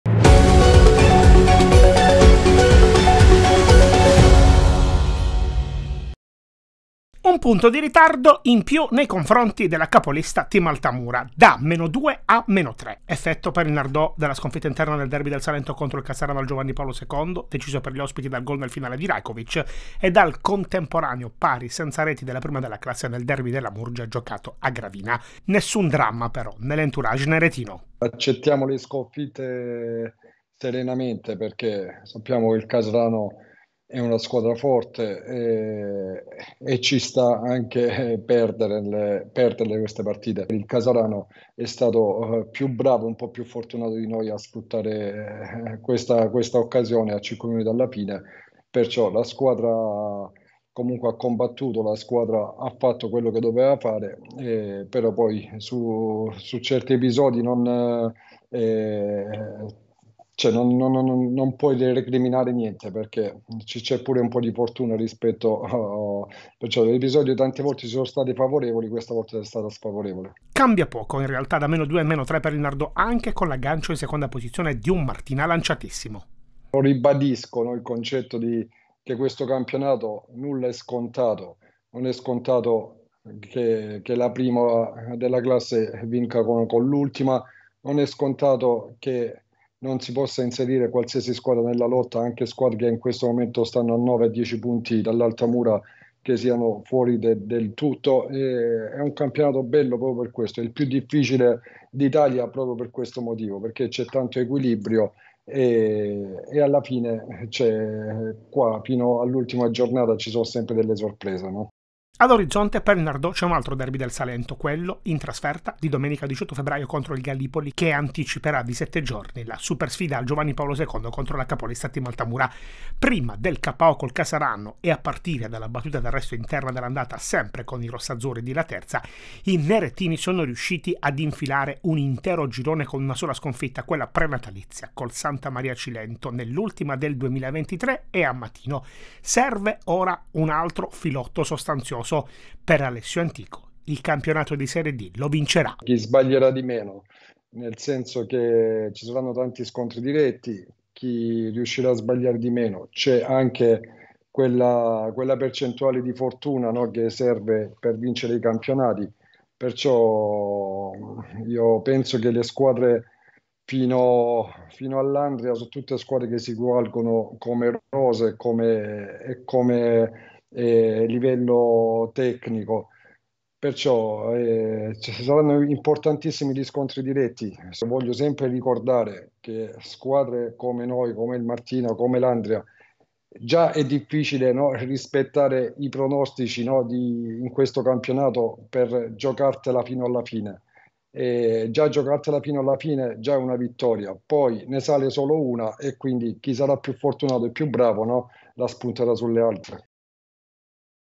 NARDO’ | «scivola» a -3 dalla vetta
Antico: «Nessun problema, vincerà il campionato chi sbaglierà di meno»